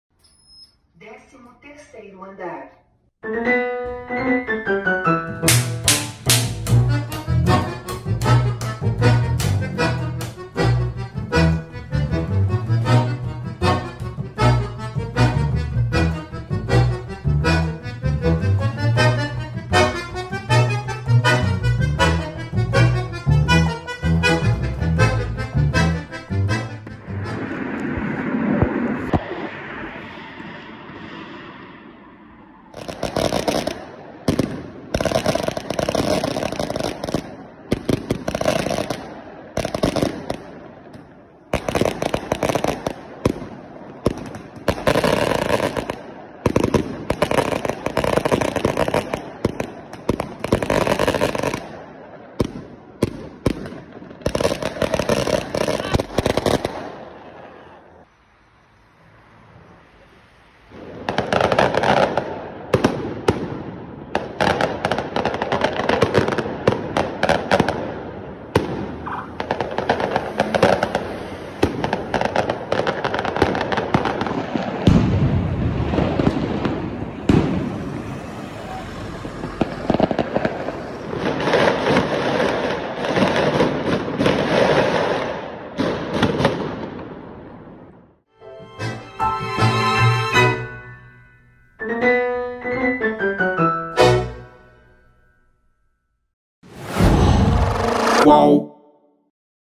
Torcida do Atlético-MG faz foguetório em hotel do Boca antes de decisão
Torcedores do Atlético-MG soltaram fogos de artificio no entorno do hotel em que o Boca Juniors-ARG, adversário do alvinegro nas oitavas de final da Libertadores, ficou em Belo Horizonte. O UOL se hospedou em um hotel em frente ao do Boca e acompanhou a movimentação dos atleticanos.

A ação, chamada nas redes sociais de "réveillon antecipado", começou às 23 horas de segunda-feira e foi contida pela polícia por volta de 1 da manhã desta terça (20), dia do confronto pelas oitavas de final da Copa Libertadores --marcado para 19h15 (de Brasília) no Mineirão. O primeiro jogo terminou empatado em 0 a 0.